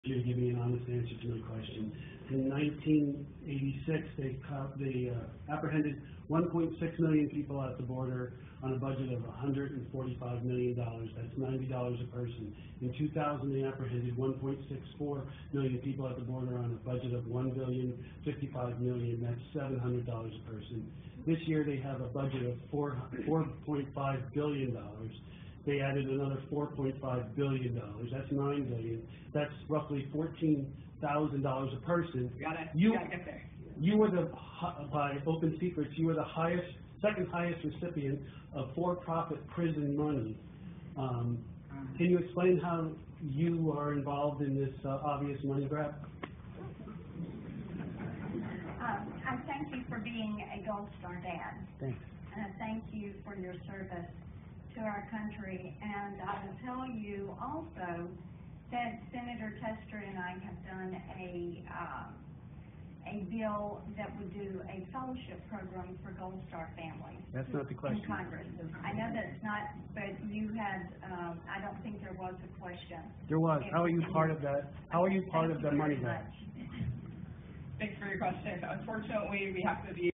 Senator Marsha Blackburn evades Gold Star Dad's question
Senator Marsha Blackburn disrespects Gold Star Dad's question about her involvement in border and for profit prisons money grab. She ignores the question and runs out of the room.